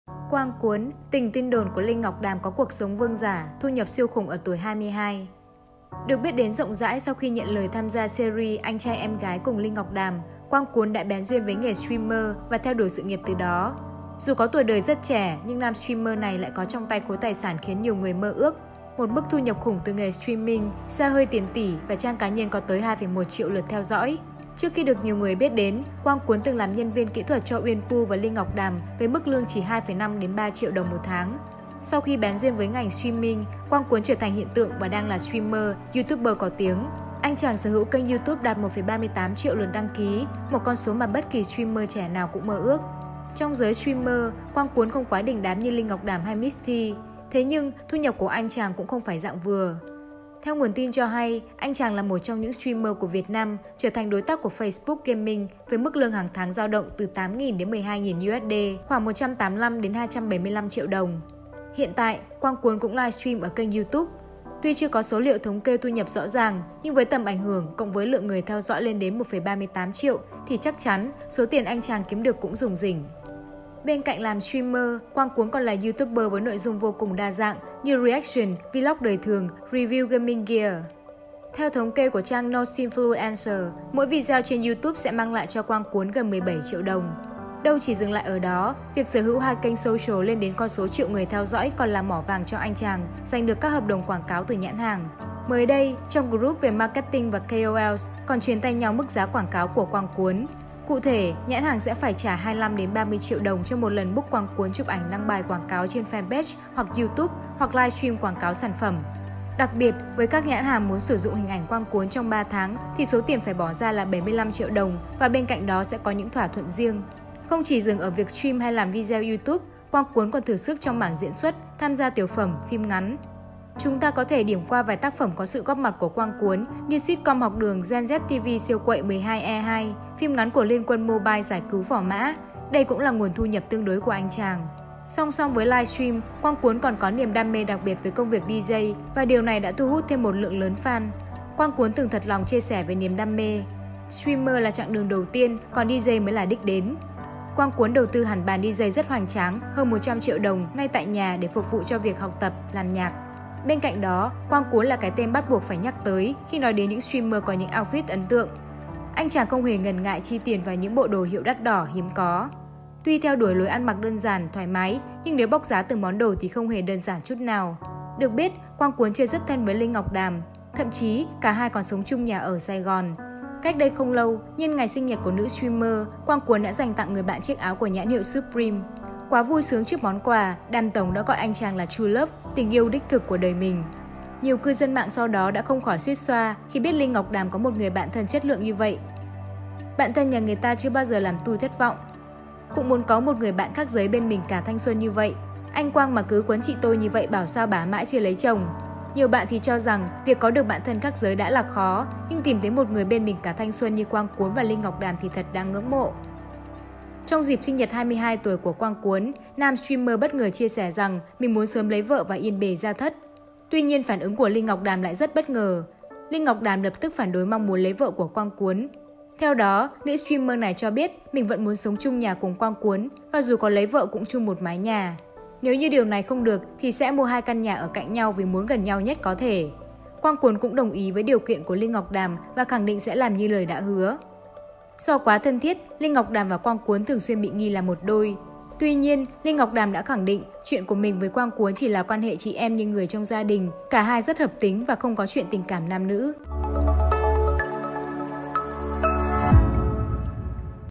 Quang Cuốn: Tình tin đồn của Linh Ngọc Đàm có cuộc sống vương giả, thu nhập siêu khủng ở tuổi 22
Được biết đến rộng rãi sau khi nhận lời tham gia series Anh Trai Em Gái cùng Linh Ngọc Đàm, Quang Cuốn đã bén duyên với nghề streamer và theo đuổi sự nghiệp từ đó. Dù có tuổi đời rất trẻ nhưng nam streamer này lại có trong tay khối tài sản khiến nhiều người mơ ước, một mức thu nhập "khủng" từ nghề streaming, xe hơi tiền tỷ và trang cá nhân có tới 2,1 triệu lượt theo dõi.